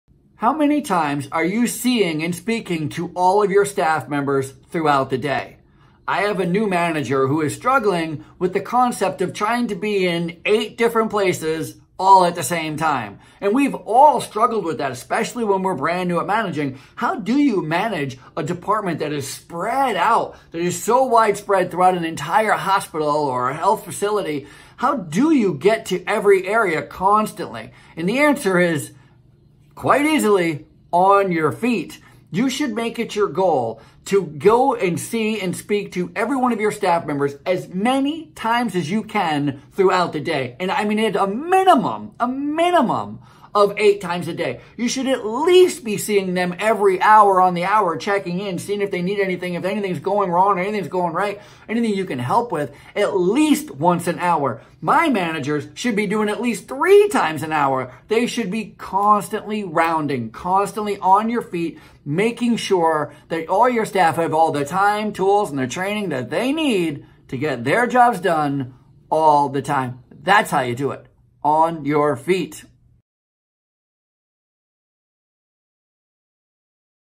ManagementIn60 Episode 60| On Foot Management| Ralph Peterson
In today's video, I'll share how you can check on your team more often throughout the day.

Please share with your teams and anyone who will benefit from watching this.